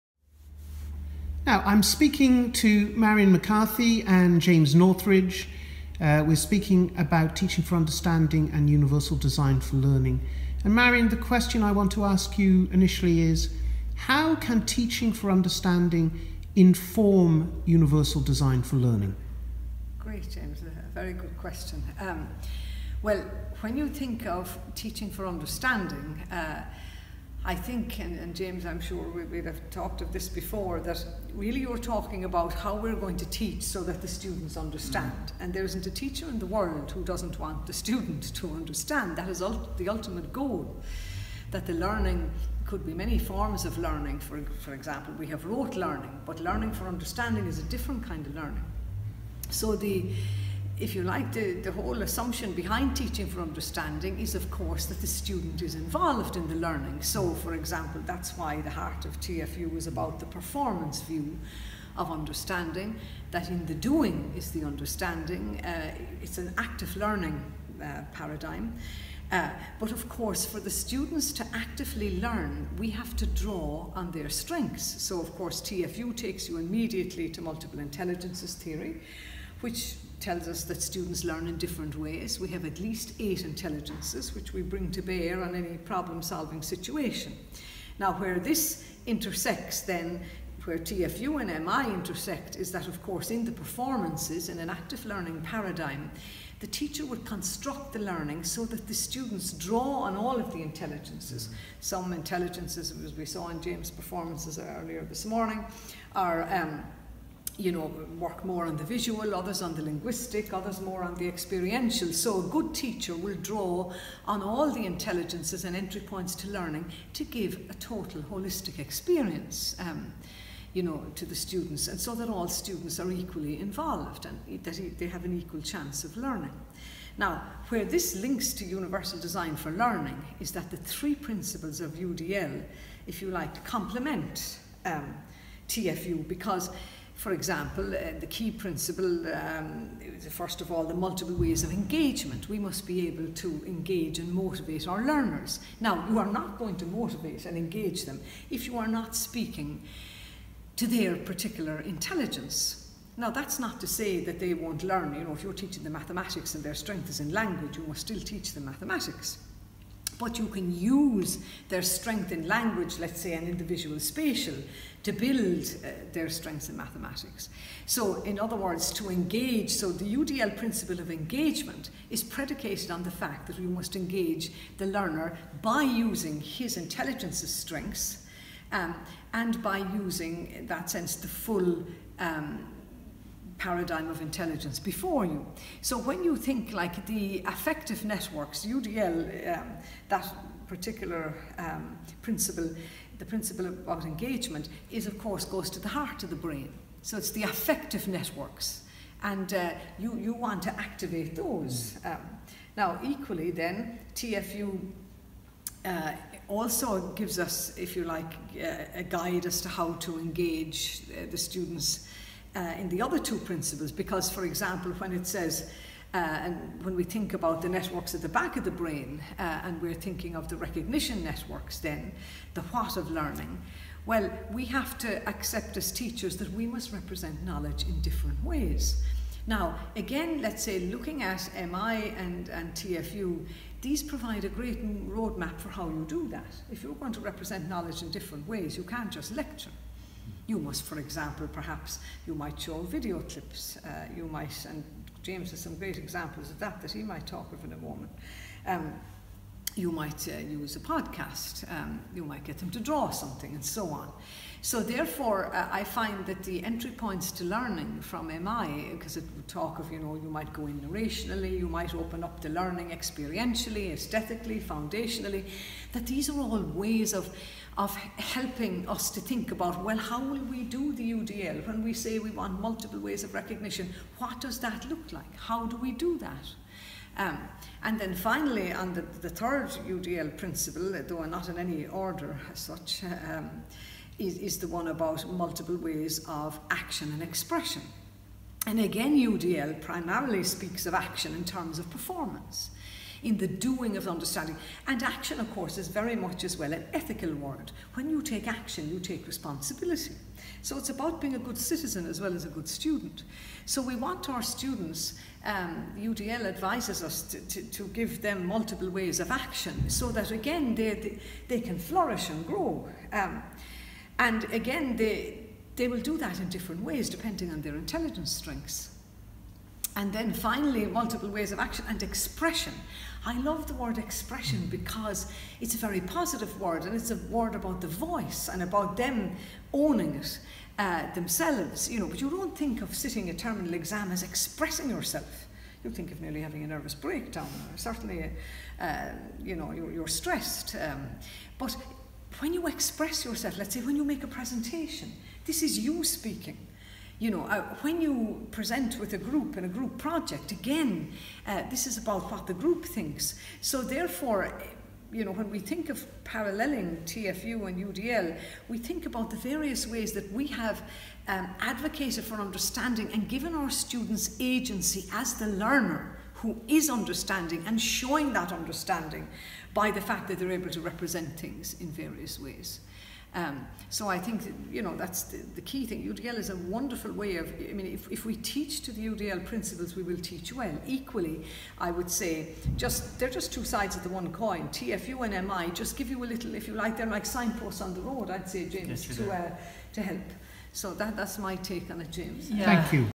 Introducing Universal Design for Learning
Dr Marian McCarthy and Marie Curie Fellow James Northridge on UDL